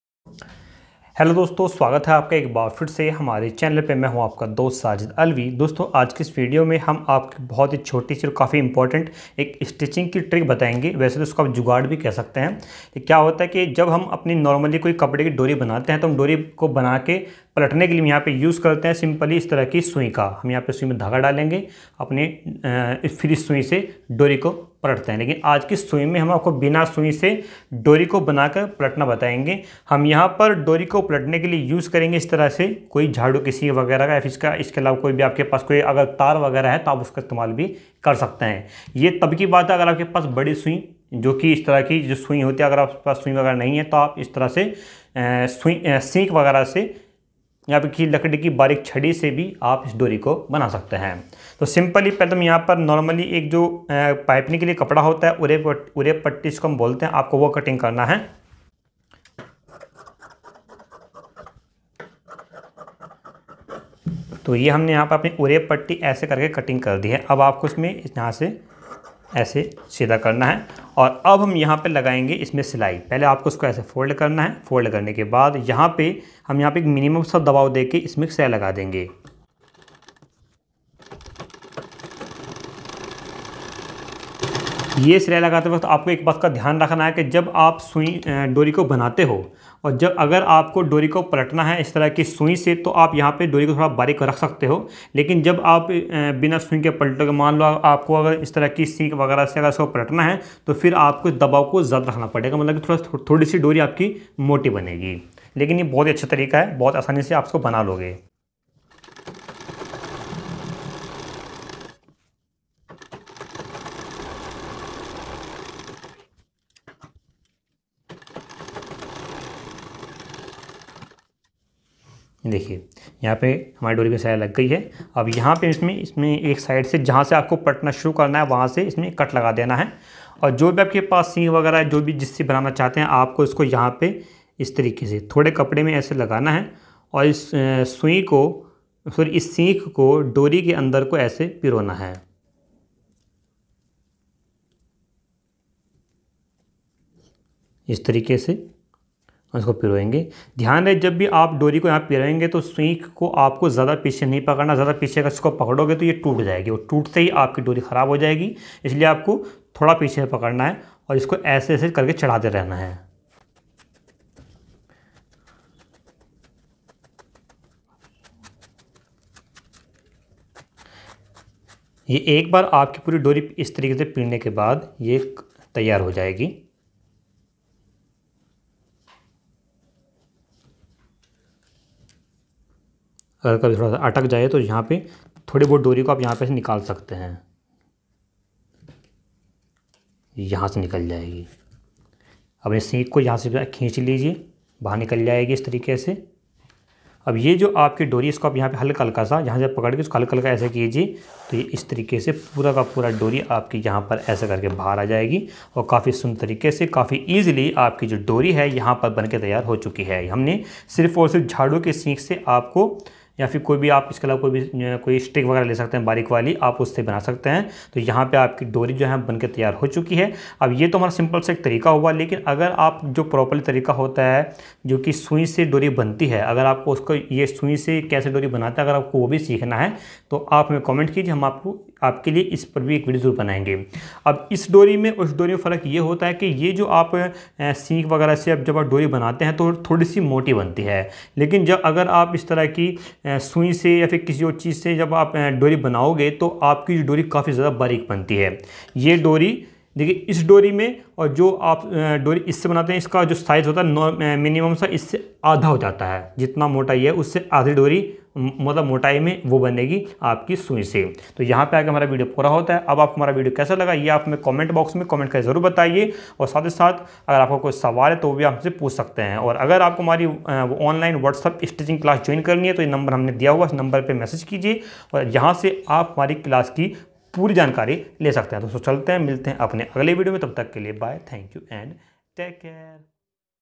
झाड़ू की सिंक से डोरी केसे बनाते हैं हम आप को आज सिखाएँगे/How to make dori to broom
सिलाई की ऑनलाइन क्लास के लिये हमारे whatsapp नम्बर पे सम्पर्क किजीये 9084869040

(Fees Rs.2000) ful Course

Contact Online classes only